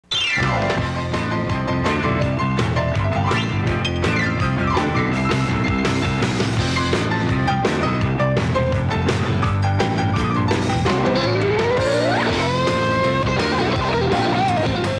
Life Savers candy commercial 1990
A very musical 15 second commercial for Life Savers candy which aired on Canadian television in 1990.